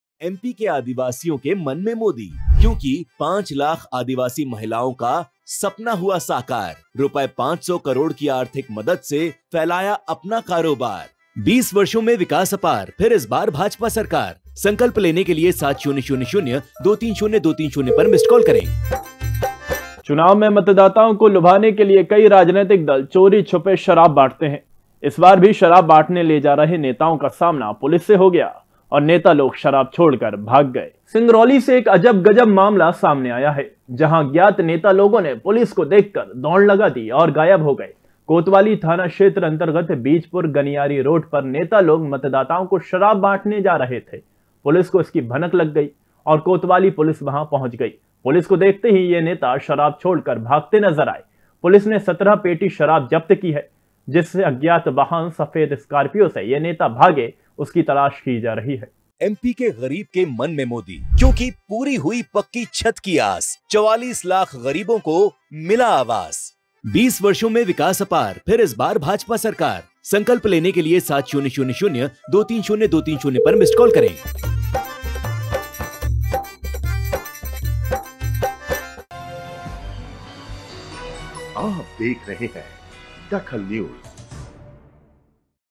DAKHAL NEWS पुलिस को देखकर नेता दारू छोड़कर भागे
पुलिस को देखकर नेता दारू छोड़कर भागे
नेता लोग चुनाव में बांटने ले जा रहे थे शराब